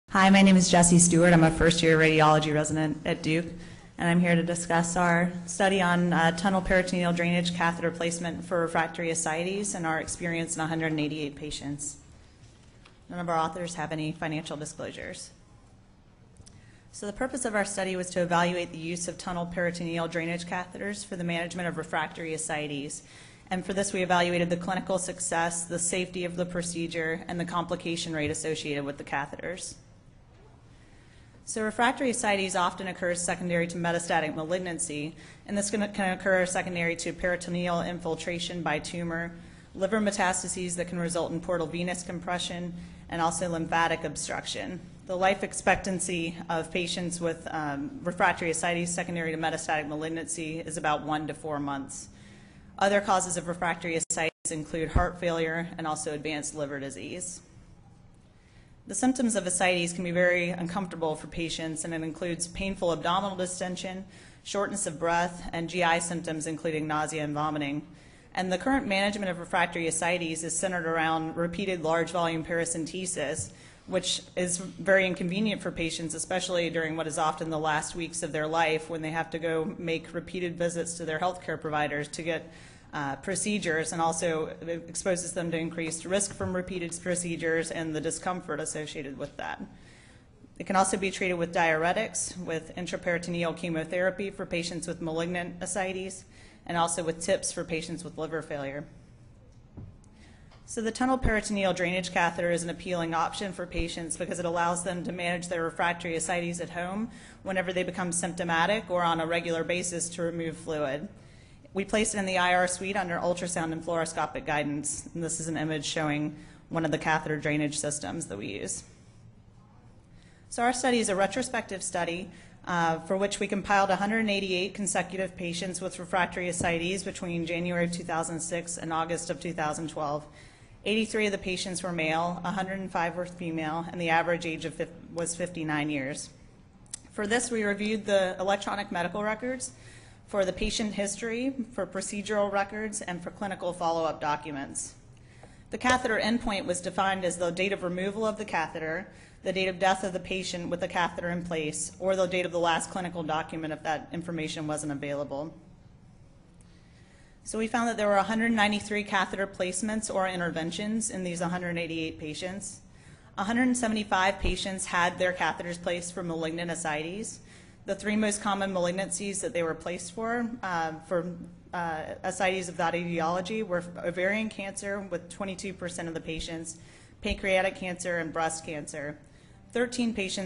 Tunneled Peritoneal Drainage Catheter Placement for Refractory Ascites
JK Stewart, MP Lungren, CY Kim, et al
Duke University Hospital
Durham, NC, USA

Presented at SIR 2013

to watch the complete video and see other clinical videos